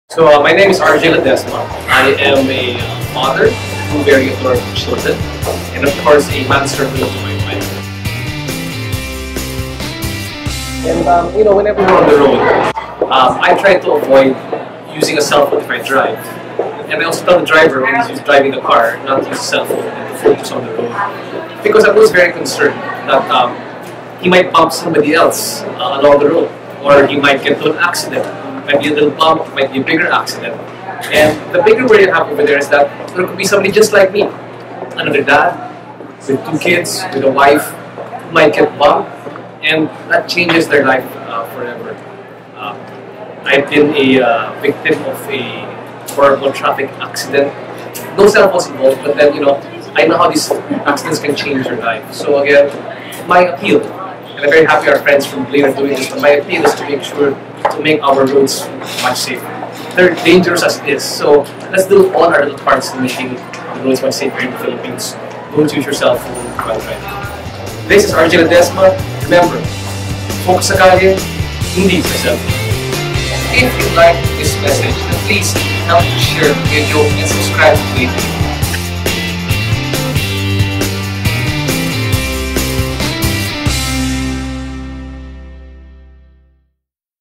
Focus Sa Kalye, Hindi Sa Cellphone : RJ Ledesma
"I've been a Victim of a Horrible Traffic Accident."  -  RJ Ledesma.

On any given day, RJ Ledesma is hosting an event, taping a segment for his TV show, overseeing the operations of his food markets, and writing his weekly column.

In this video, RJ shares his personal experience and warns our viewers about the dangers of using mobile phones while driving.